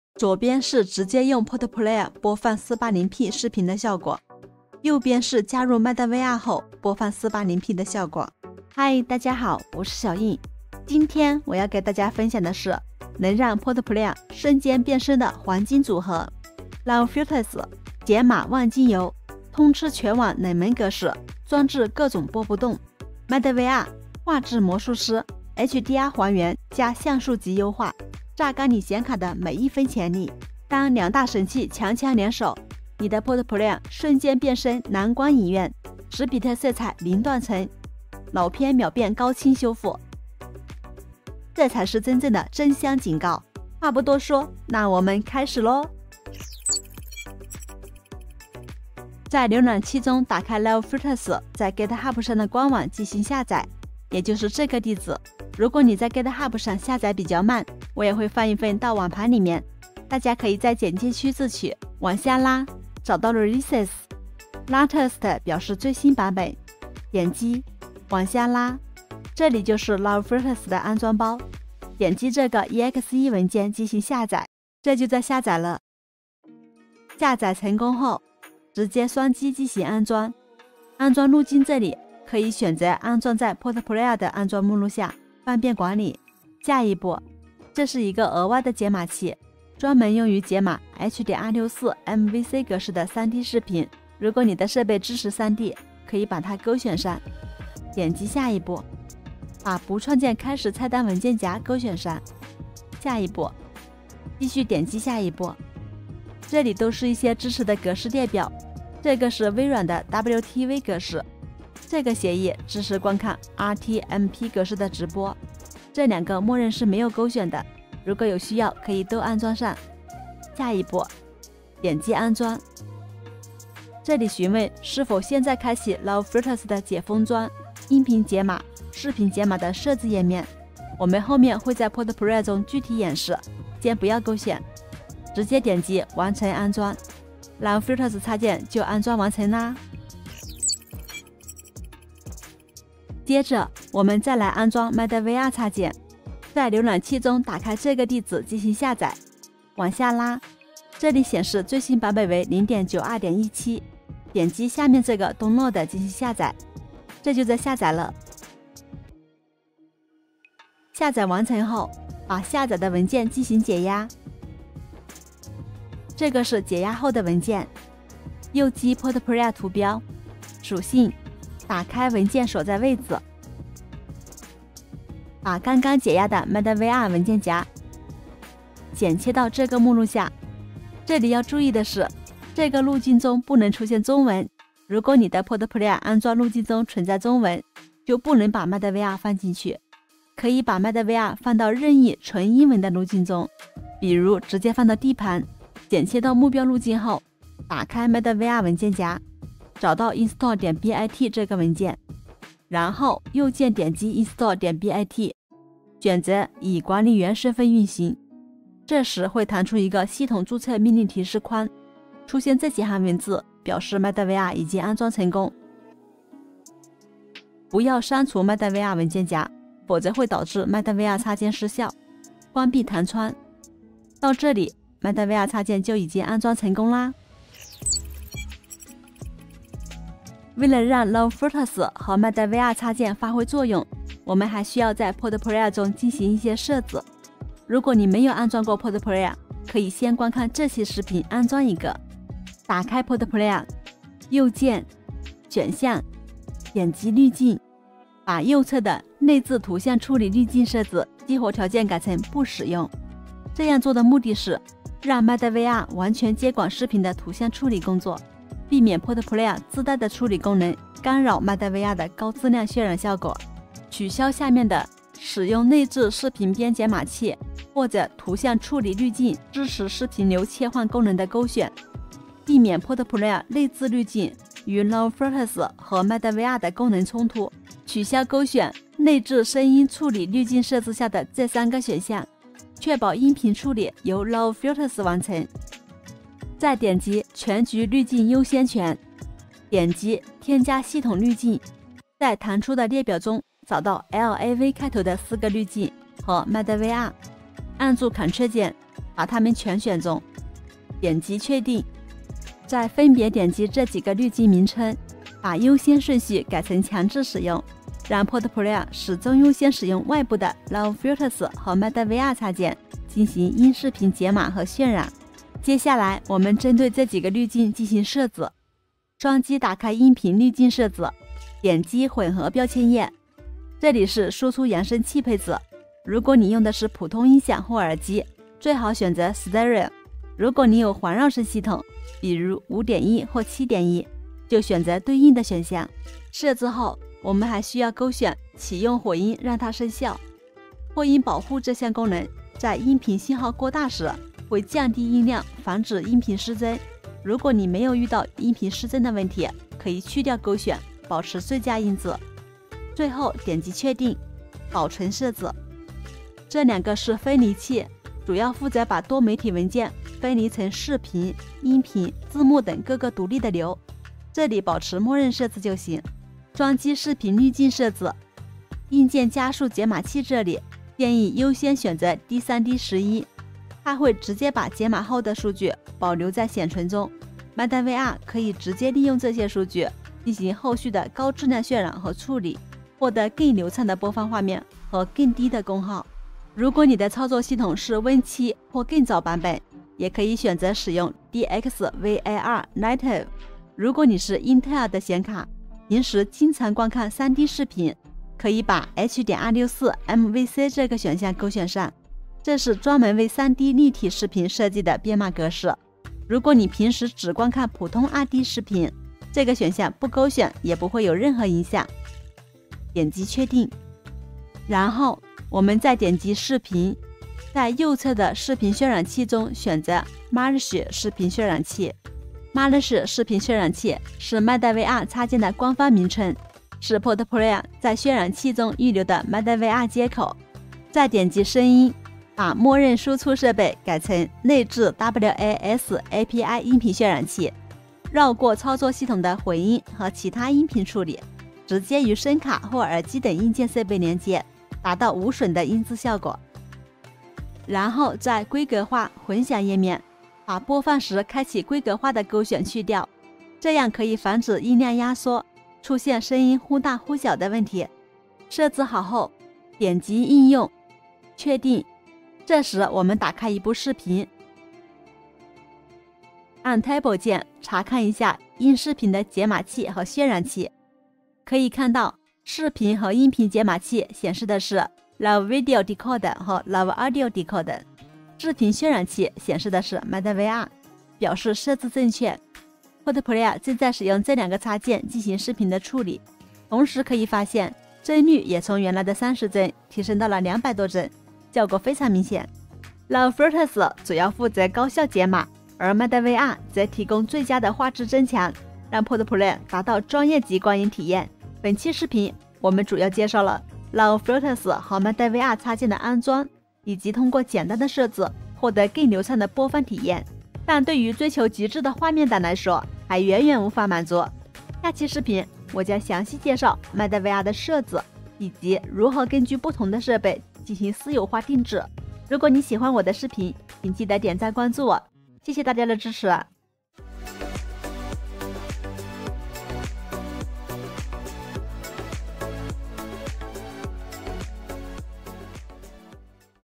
【madVR全网最全攻略】potplayer终极调教|LAV Filters+madVR榨干显卡性能|打造家庭影院天花板！
在今天的视频中，我将向大家展示如何通过LAVFilters和madVR这两款插件，让你的PotPlayer播放视频更流畅、更清晰！
通过安装和简单的设置，你将能体验到更高质量的解码和画质优化。
低画质的视频在加入madVR后，画质立马提升，色彩和细节更加丰富，给你一种全新的观影体验。
由于视频内容过长，我将视频分为了上、下两期，下期视频我将详细解释madVR的设置。

如果你觉得这个视频对你有帮助，别忘了“👍点赞”和“❤订阅”我的频道，这样你就不会错过接下来的教程了。同时，如果你有任何问题，请在**评论区**留言，我会尽快回复你。

0:00 前言
0:47 LAV Filters插件安装
2:21 madVR插件安装
3:48 PotPlayer基础设置